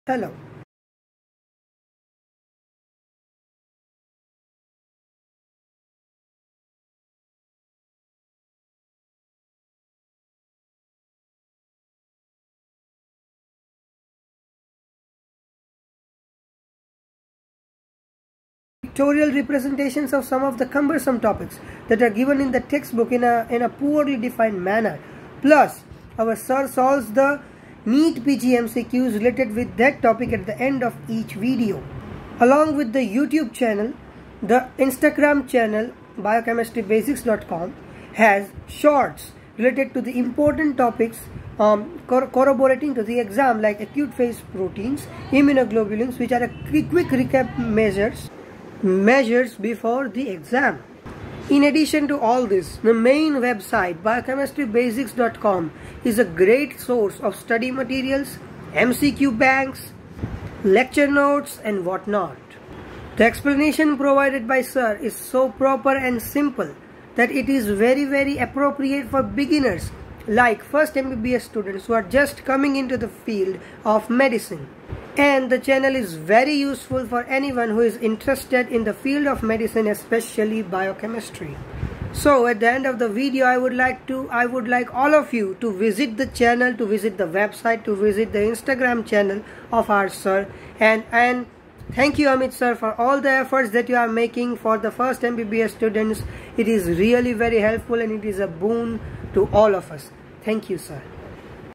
Student Testimonial Part 4 - Biochemistry Basics By Dr Amit
This is the student testimonial video for youtube channel biochemistry basics by dr amit explaining how it helped them in studying biochemistry and in concept building